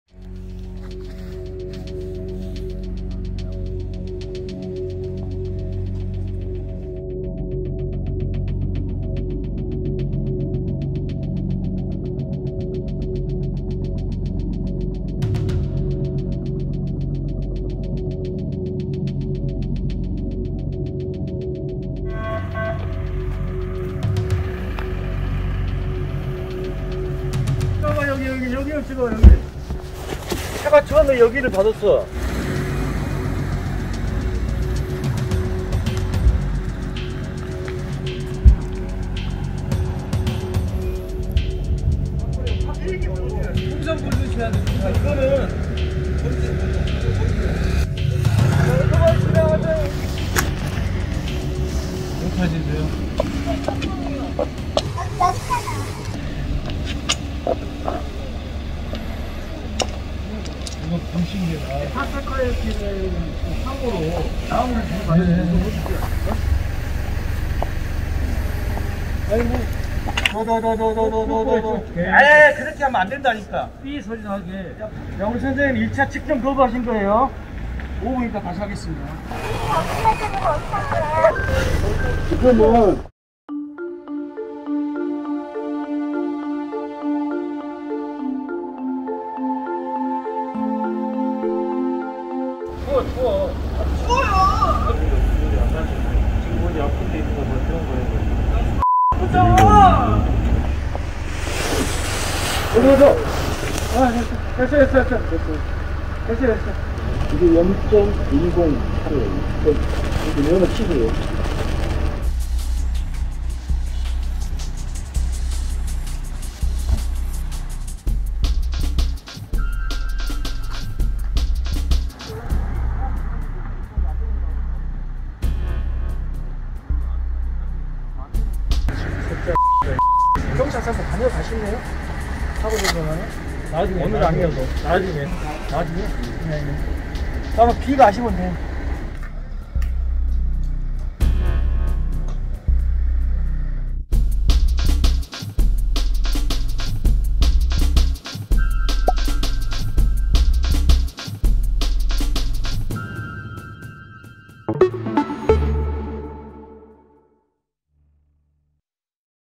(단독_풀영상) 미스코리아 서예진 만취해 나무와 충돌…현장에서 면허 취소 / 비디오머그
미스코리아 출신 방송인 서예진씨가 어젯밤 강남에서 술에 만취한 채로 차를 몰다 가로수를 두 차례나 들이받는 사고를 냈습니다. 음주측정에서 혈중알콜농도 0.108을 기록해 면허 취소 처분을 받았습니다.